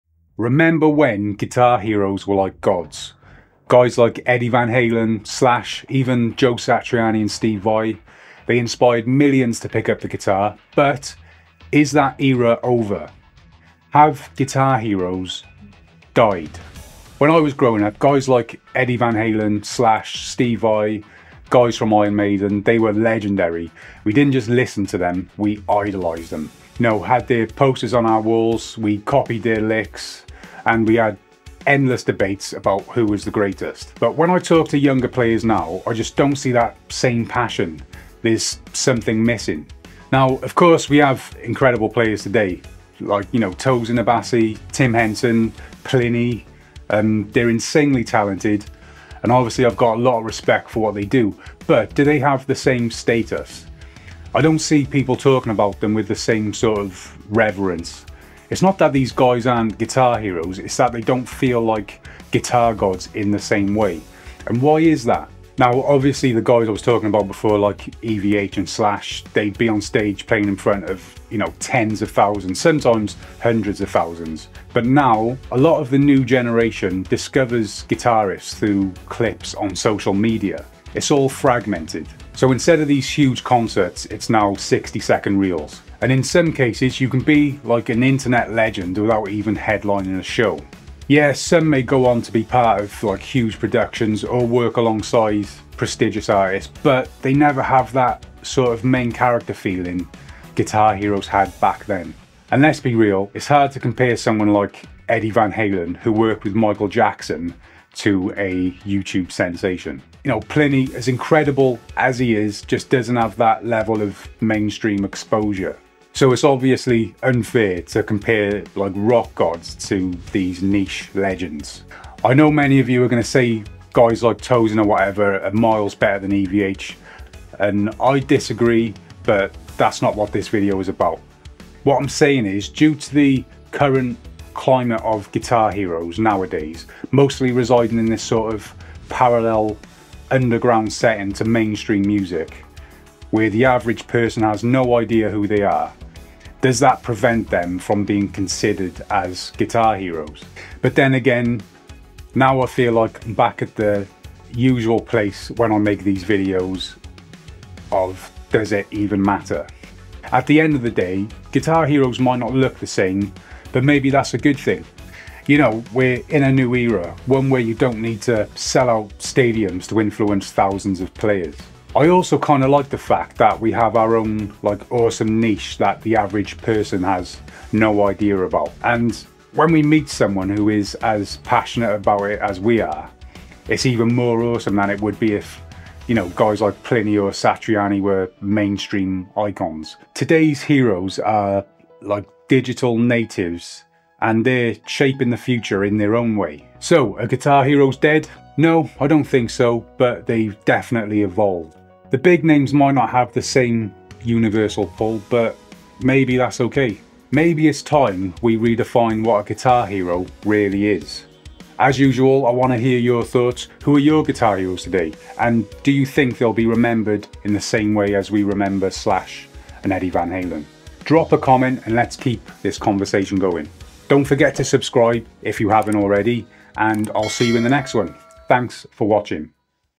The TRUTH about Today's Guitar Legends
Are we witnessing the end of the guitar hero era? Once icons like Slash, Eddie Van Halen, and Joe Satriani ruled the stage, but who are today's legends? In this video, we dive deep into the evolution of guitar heroes, from the glory days of rock to the rise of modern virtuosos like Tim Henson and Tosin Abasi. Are guitar heroes still inspiring the masses, or is this a relic of the past? Let's talk about it! ⚡🎶guitarhero

00:00 - Intro
00:39 - Modern Guitar Heroes
01:05 - Old vs New
01:53 - Why I don't compare old vs new
02:36 - Does it even matter?
03:16 - Are guitar heroes dead?

LINKS